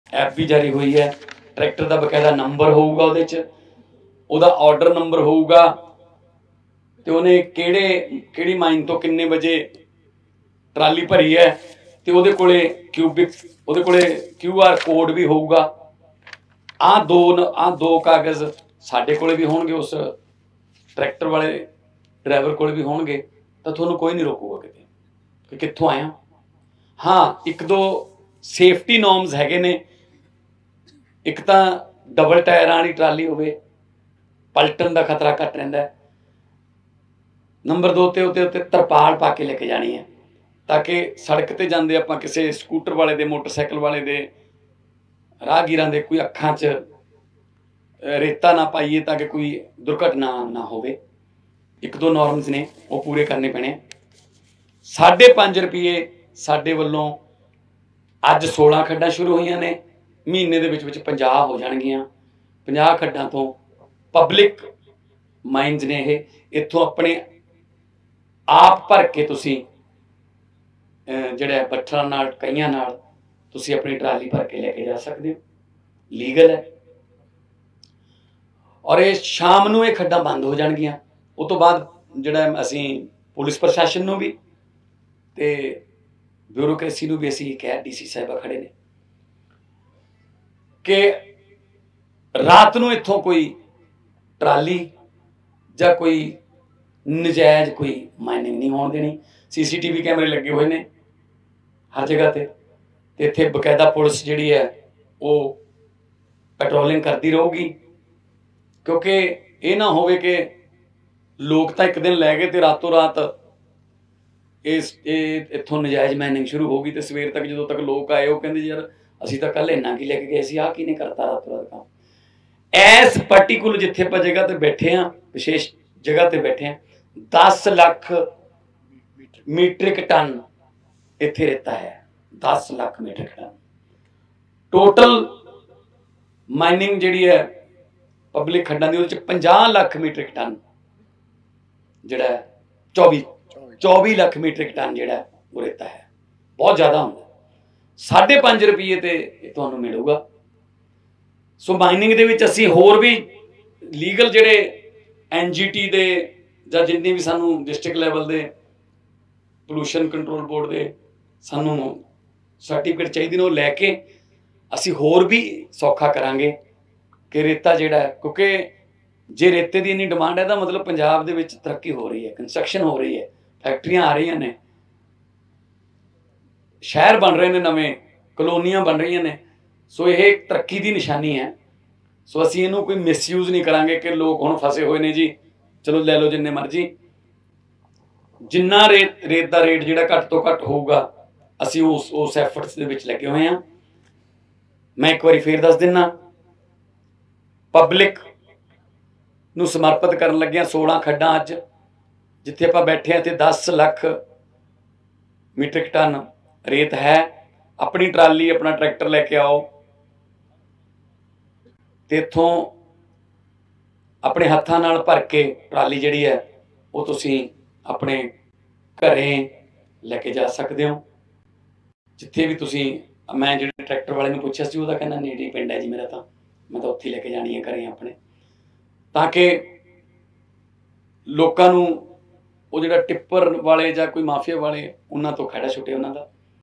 ਰੇਤਾ ਦੀ ਖੱਡ ਲੱਭਣ ਲਈ ਮਾਨ ਸਰਕਾਰ ਨੇ ਜਾਰੀ ਕੀਤੀ ਖਾਸ app, ਸਾਰੀ ਜਾਣਕਾਰੀ ਮਿਲੇਗੀ ਫੋਨ 'ਤੇ | Punjabi Bulletin
Punjabi Bulletin